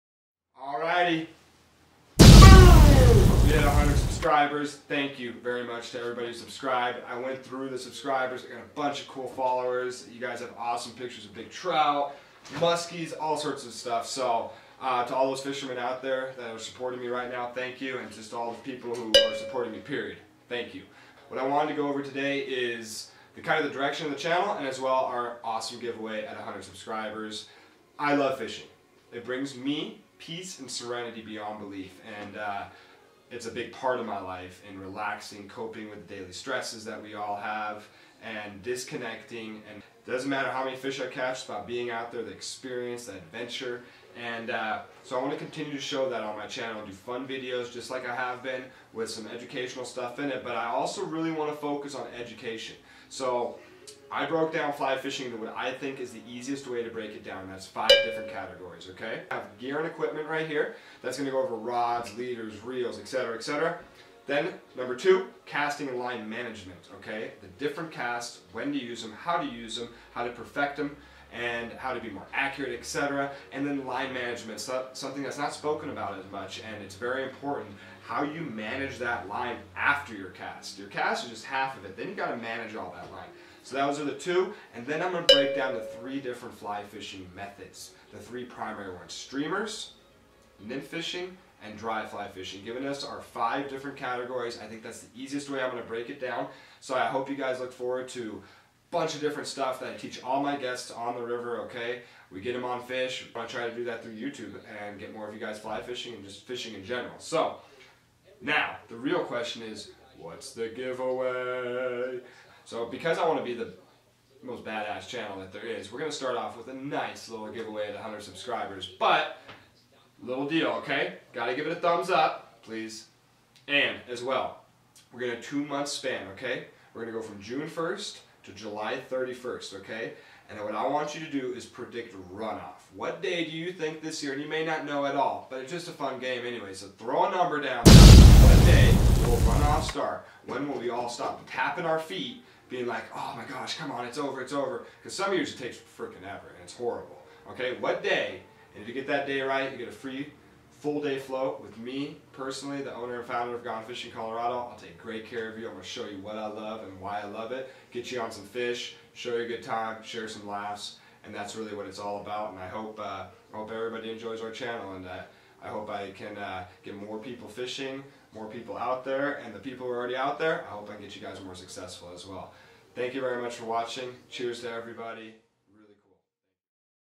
100 subscriber giveaway FREE FLOAT TRIP thank you!! (2018)
100 Subscribers giveaway FREE FLOAT TRIP! thank you!! (2018) It has been an awesome journey and i look forward to the summer! Thank you again to my first 100 you guys are all awesome. NEVER STOP FISHING